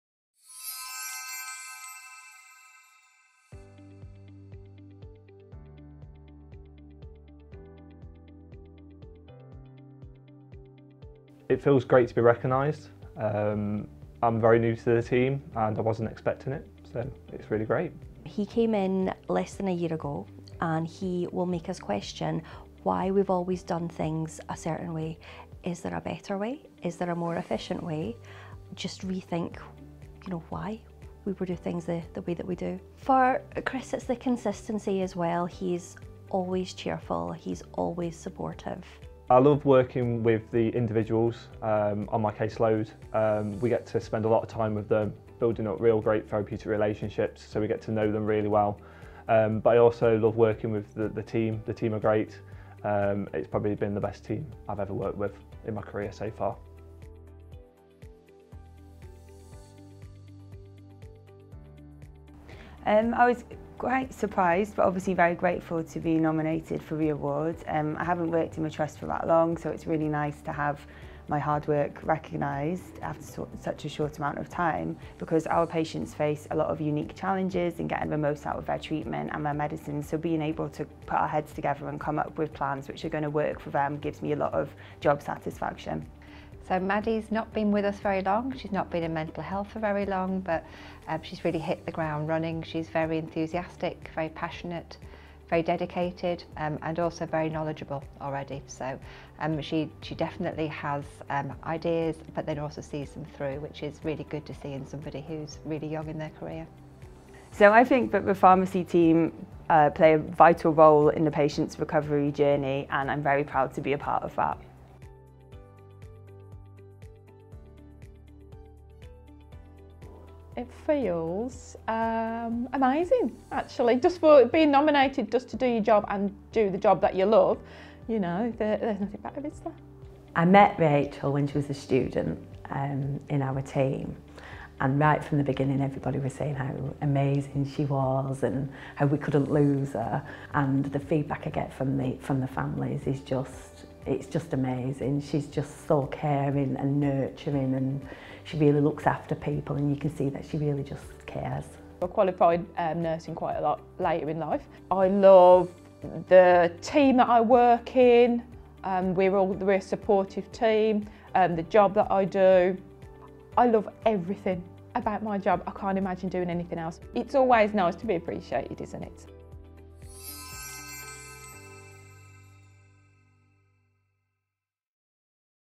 Rising star - Pennine Care People Awards 2022
We celebrated lots of incredible colleagues and stories at our PennineCarePeople Awards on 1 December 2022.

Here's a look at the rising star category. The winner was Christopher Hardy, care co-ordinator with the Heywood, Middleton, and Rochdale learning disability team